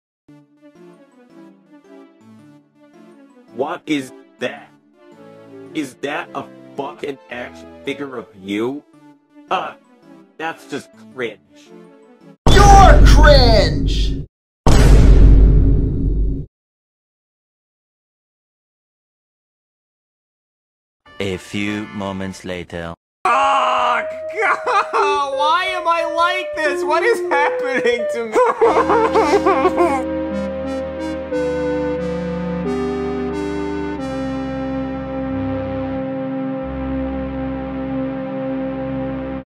Is That a Action Figure of Your OC?!? “Comic Dub” {MLP/Brony Fan Comic}
I did a comic inspired by a SpongeBob comic meme.
Geoffrey is voiced by Me (obviously)
Haze is voiced by Kits ai (Brooklyn T Guy Voice) (R.I.P Kits ai’s community library)